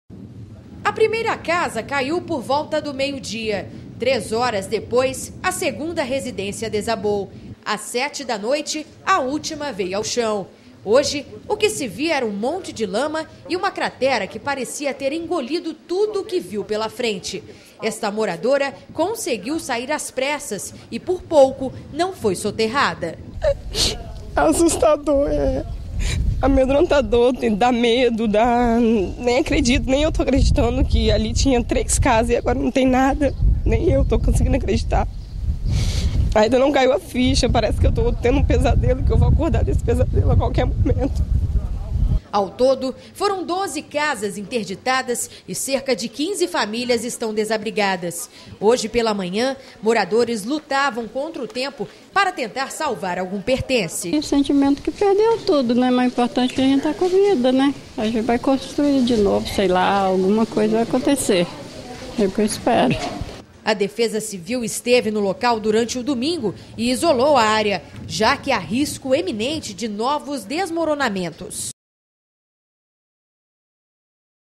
Casas desabam em Vitória
Em uma delas a família já estava retirando os móveis quando tudo veio abaixo. As principais ruas da capital ficaram debaixo d' água. Veja na reportagem de Mariana Rodrigues e Pablo Ferreira.